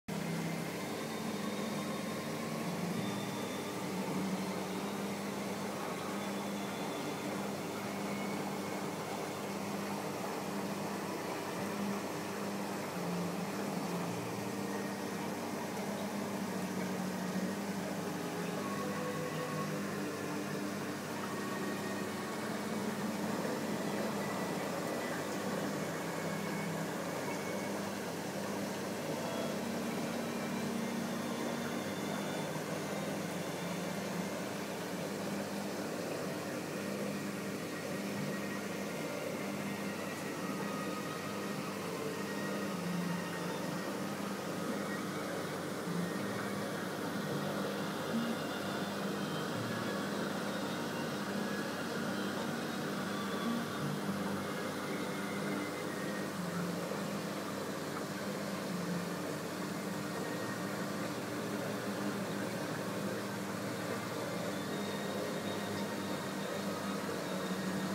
Flamboyant Cuttlefish
Flamboyant cuttlefish, Metasepia pfefferi showing it's striking characteristics as it lay up to snare a shrimp.